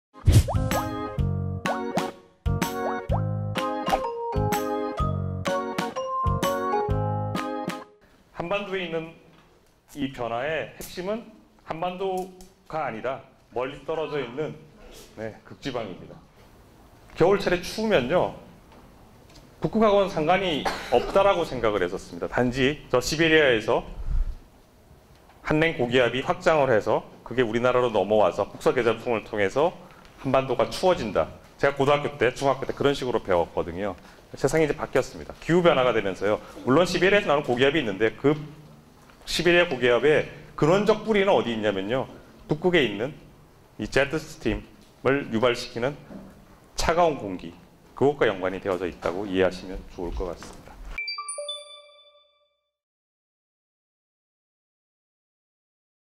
19. 11. 22 금요일에 과학터치 부산지역 29회차 하이라이트
19.11.22 부산지역
극지과학자가 들려주는 기후변화이야기 - 인하대학교 하호경 교수 센텀시티 부산디자인센터